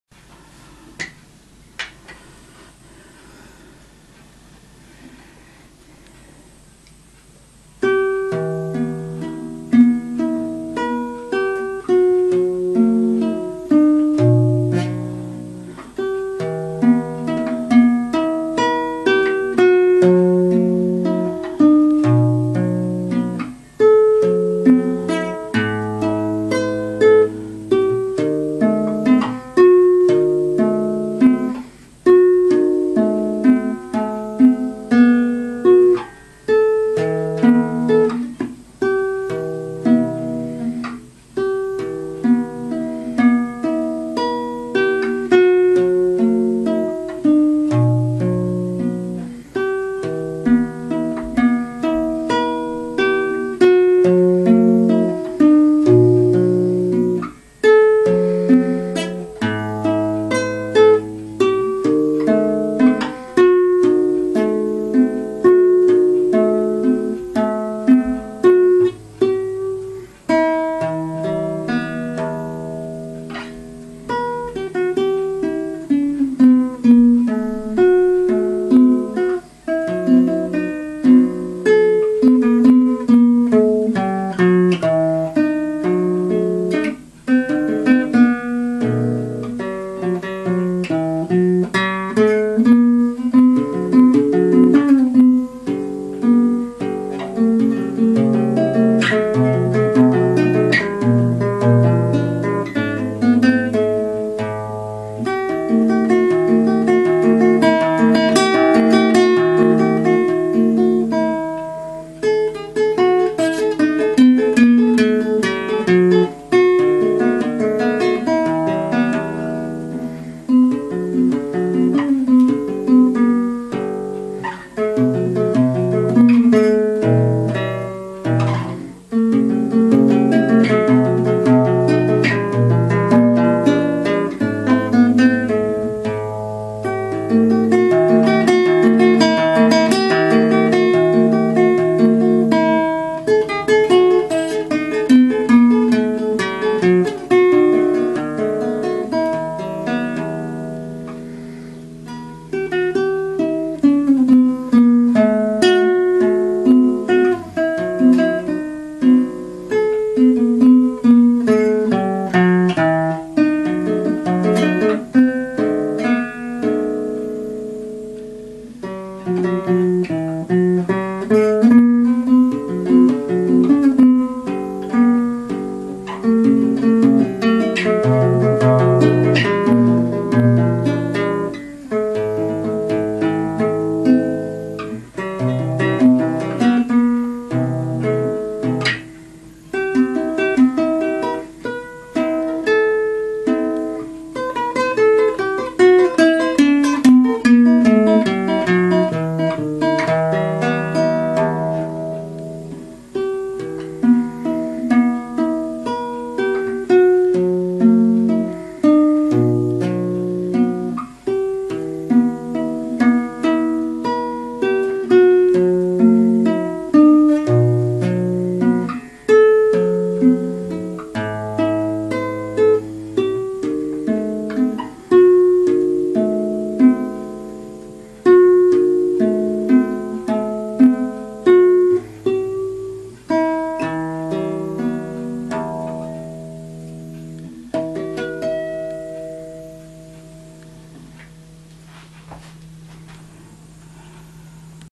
муз ю семенова=март=2015
вчера сочинил =сегодня закачал =поэтому есть шероховатости =ещё не отточил до совершенства=